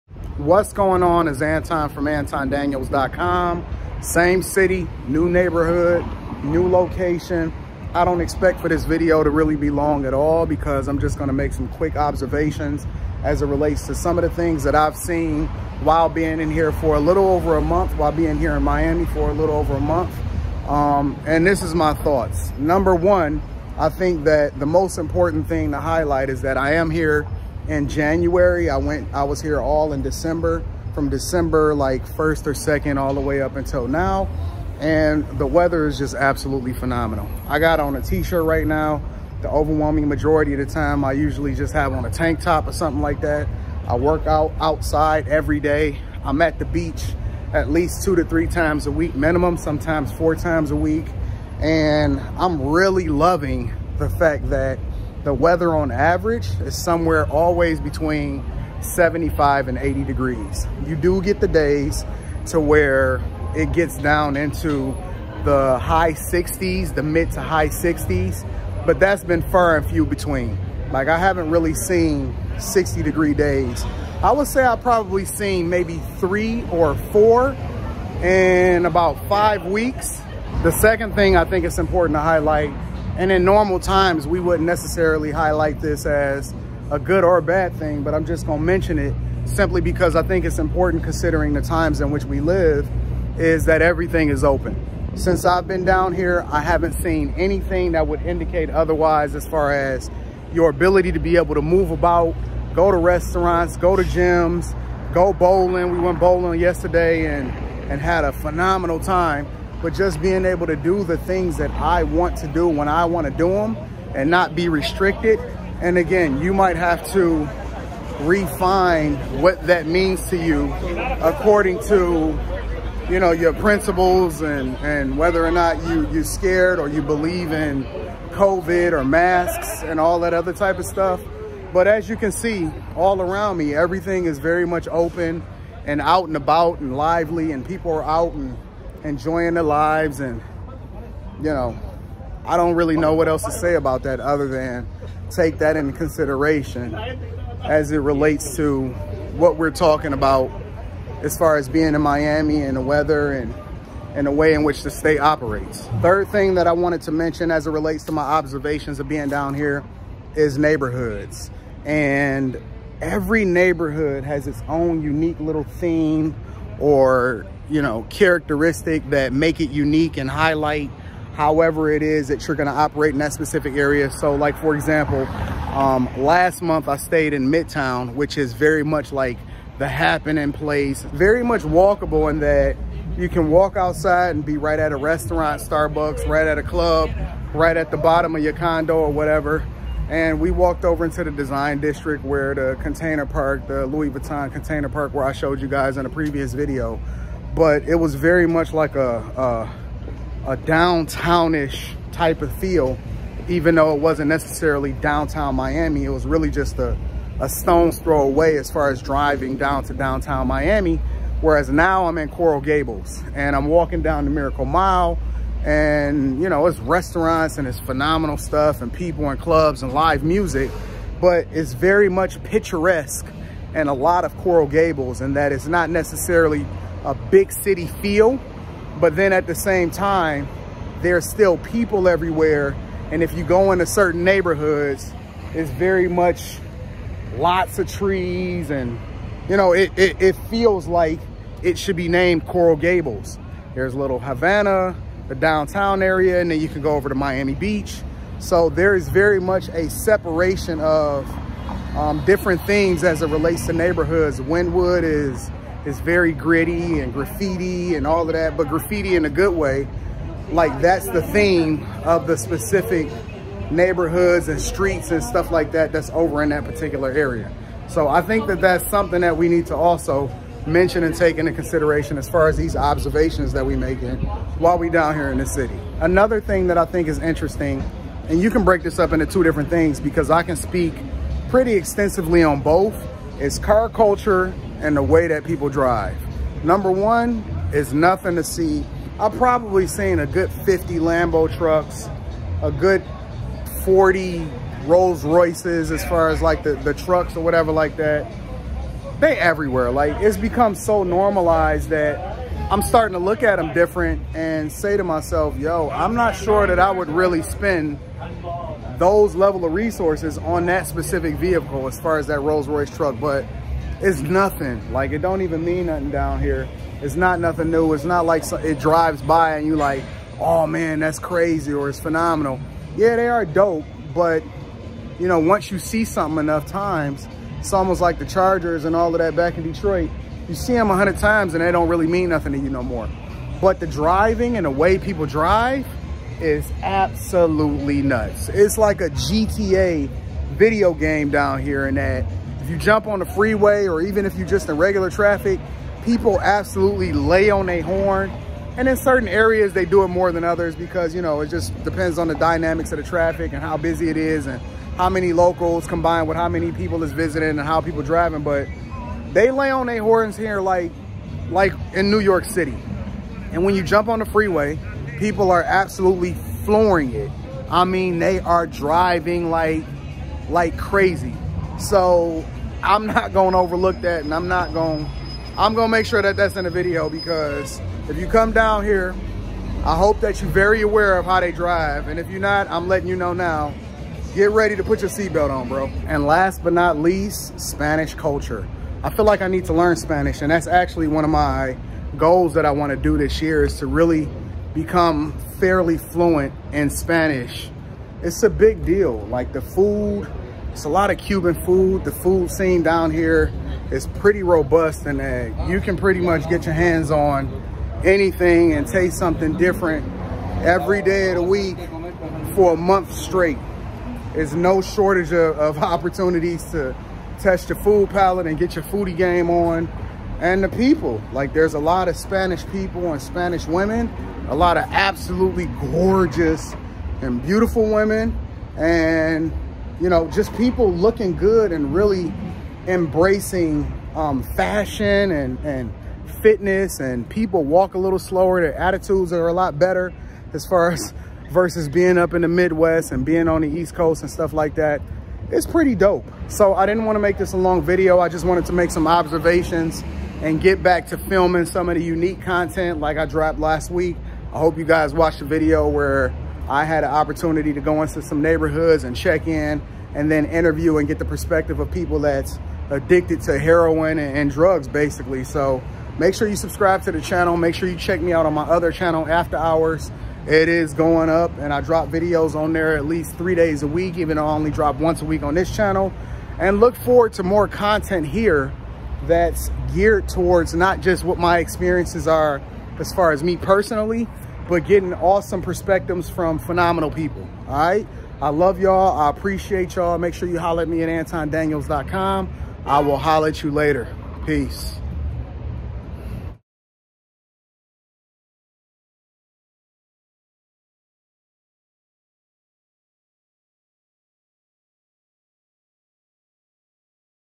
Living In Miami For Over a Month... 5 Things You Need To Consider Before Moving Here
I moved down to Miami from Detroit for the Winter. After living here for over a month, here are 5 takeaways I've observed and what you should consider before moving here.

Mail:
AntonDaniels.com